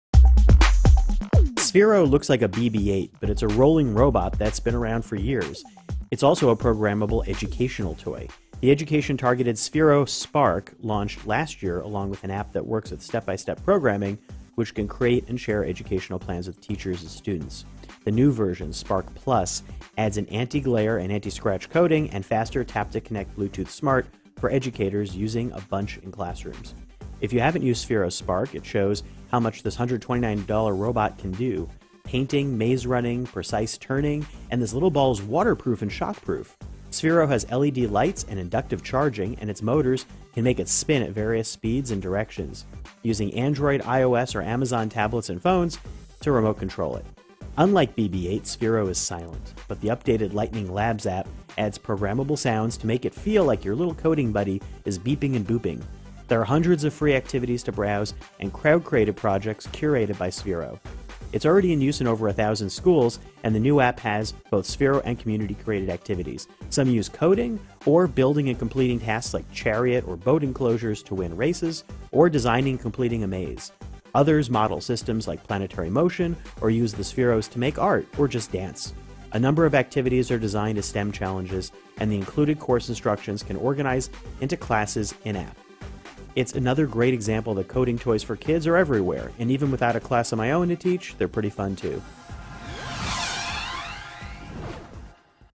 Sphero SPRK+ turns a roly-poly robot into an educational tool
From coding lessons to art projects, Sphero's new Lightning Lab app update adds extras for teachers or parents. We take a look at what Sphero can do (by the way, it's waterproof).